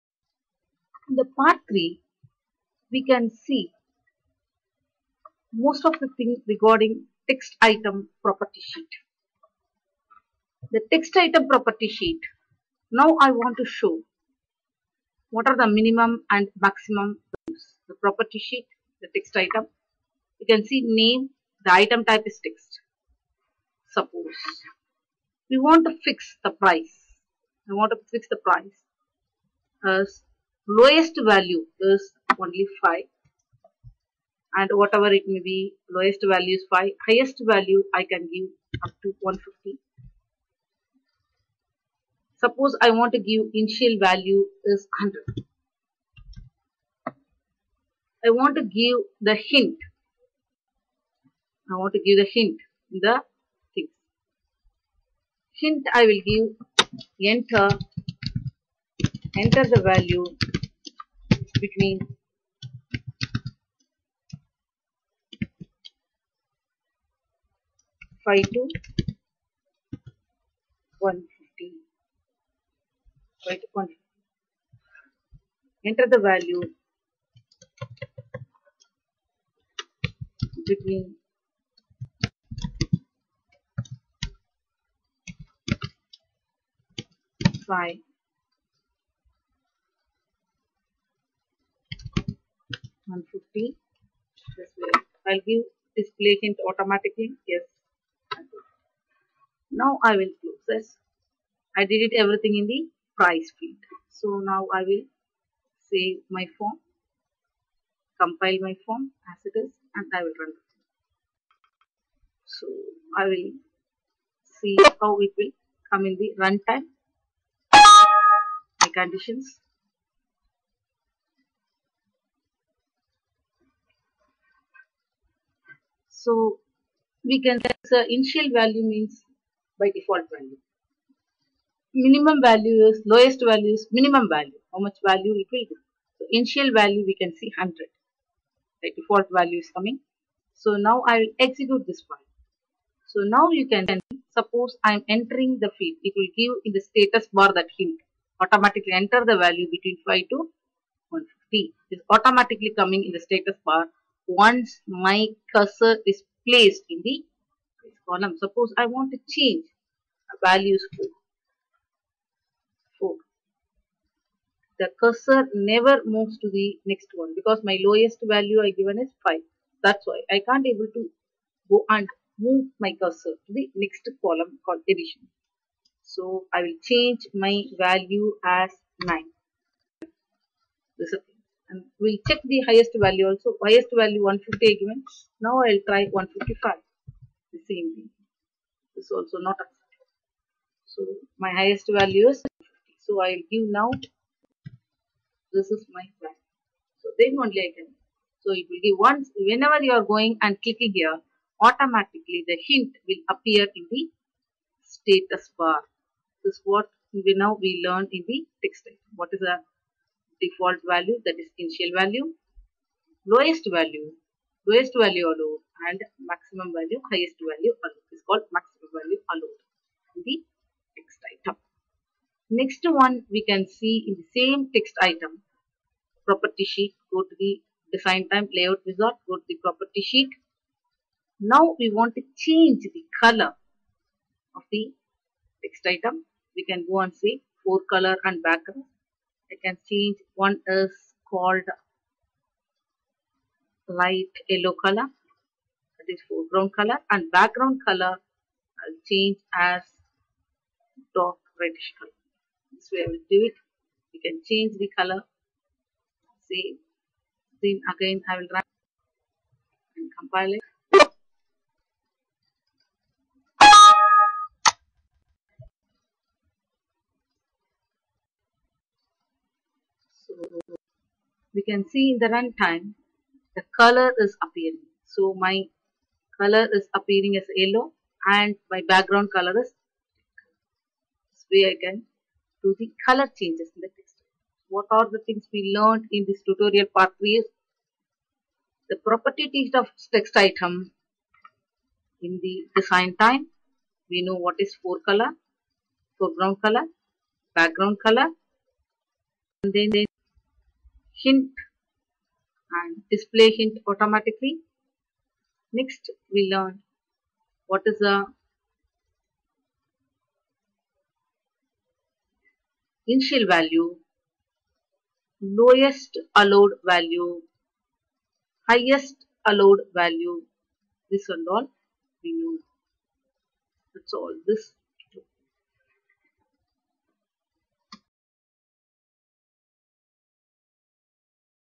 PART 3- oracle 10g form builder using text item property palette
text item property palette-lowest allowed value, highest allowed value, initial value, hint,back ground and fore color